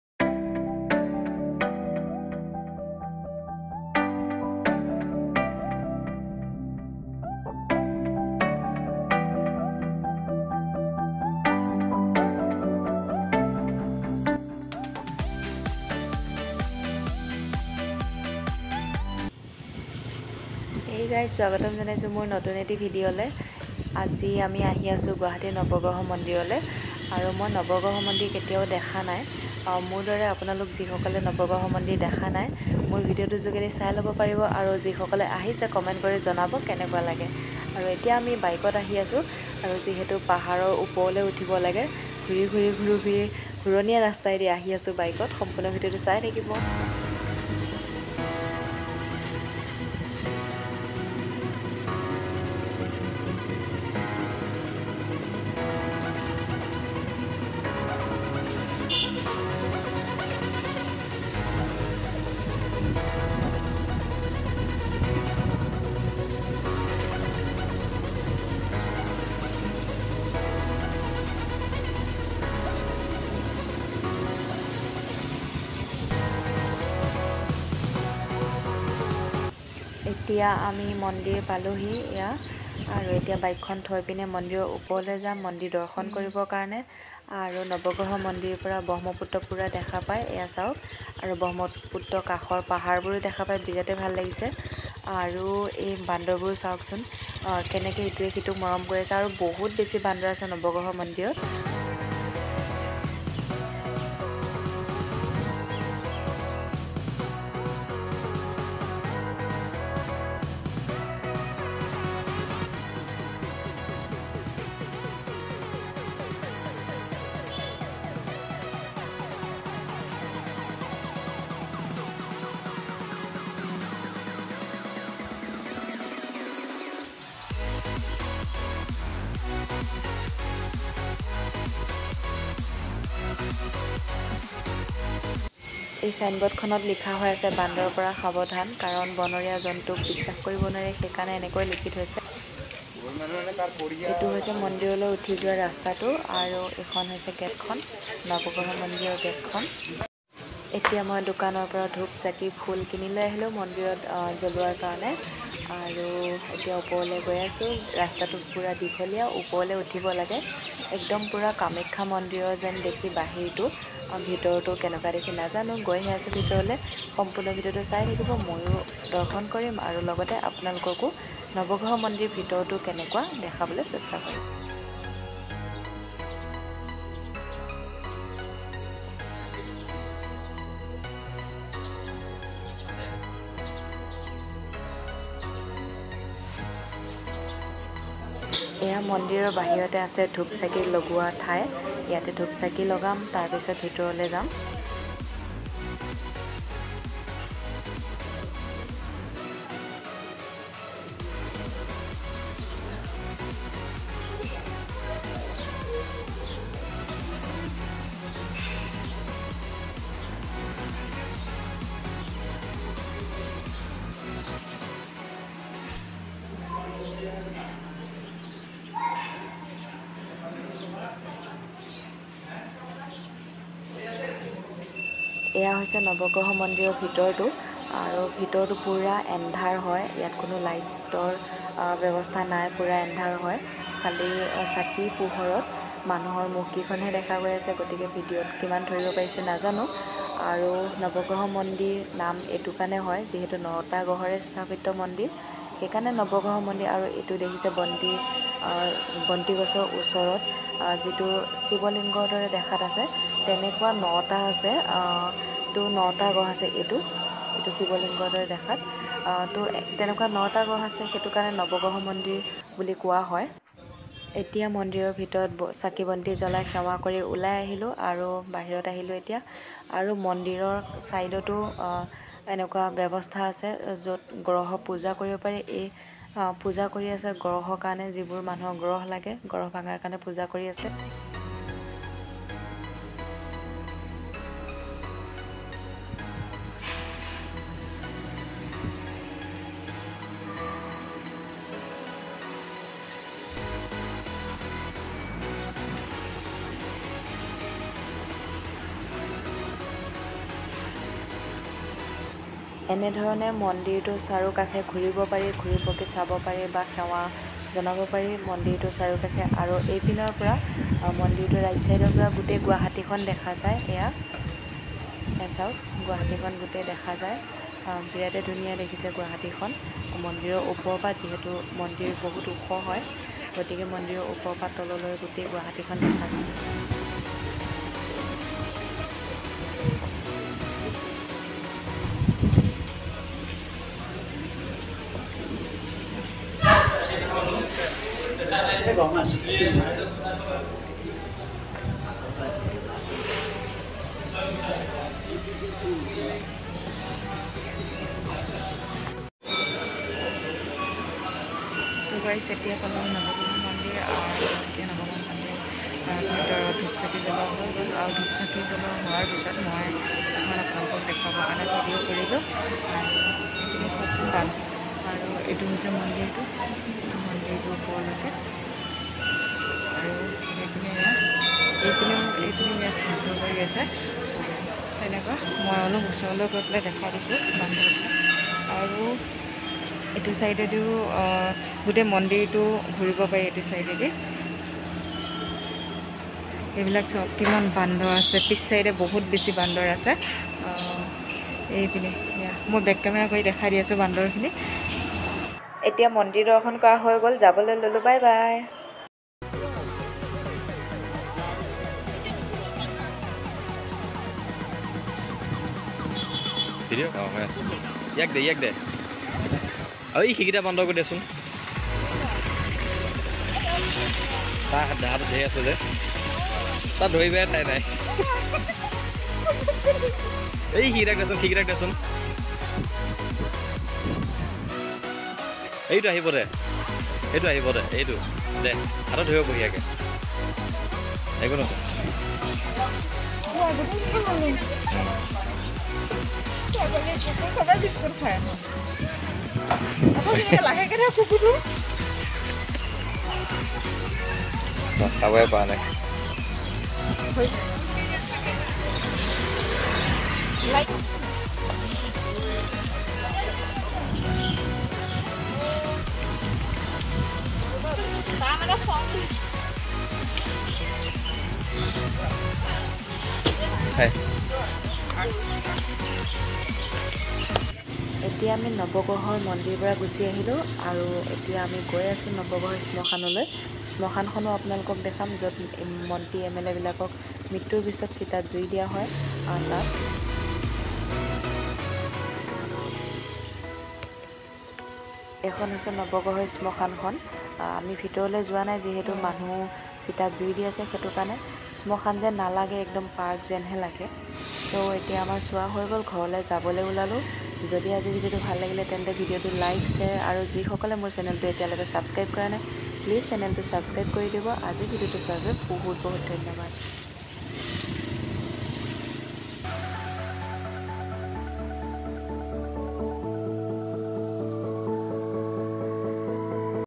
Navagraha Temple Tourism, Assam Guwahati IIনৱগ্ৰহ মন্দিৰII নৱগ্ৰহ শ্মশান গুৱাহাটী
# নৱগ্ৰহ মন্দিৰ# নৱগ্ৰহ শ্মশান গুৱাহাটীNavagraha Temple Tourism in Assam Guwahati,Chitrachal Hills

# Pooja Das Morigaon

🙏 Video টো চোৱাৰ ধন্যবাদ সকলোকে 🙏